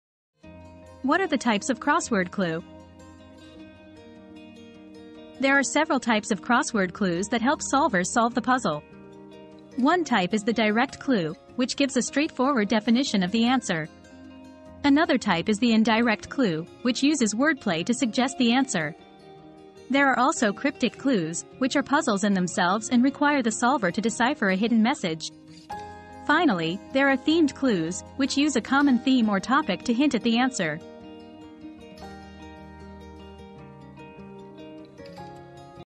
What are the types of crossword clue?
Get ready to take your crossword solving skills to the next level! In this video, we'll explore the different types of crossword clues that you need to know to become a true puzzle master. From direct clues to cryptic puzzles, we'll show you how to crack each type and reveal the hidden messages within. Whether you're a seasoned solver or just starting out, this video is a must-watch for anyone who loves a good challenge. So grab your pen and get ready to dive into the world of crossword clues!